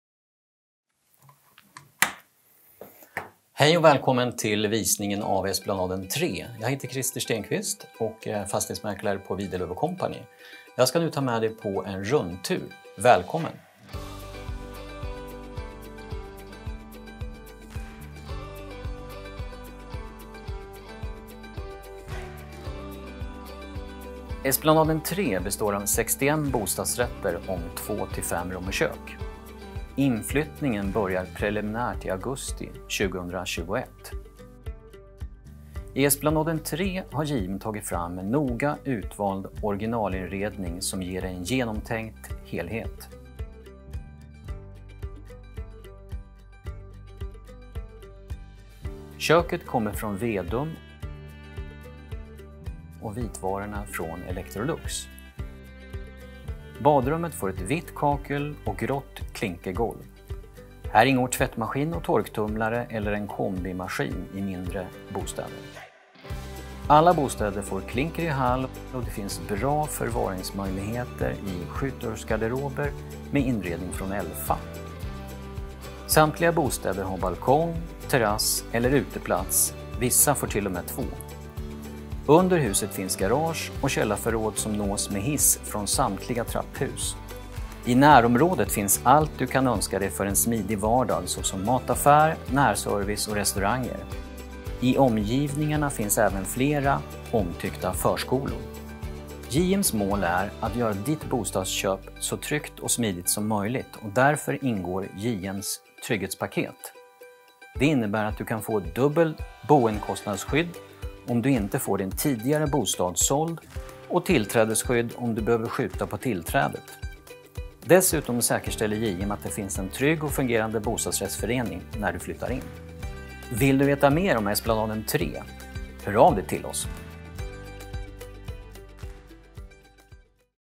Videovisning Esplanaden 3
Mäklaren Krister Stenqvist från Widerlöv & Co tar dig med på en videovisning av Esplanaden 3. Välkommen in.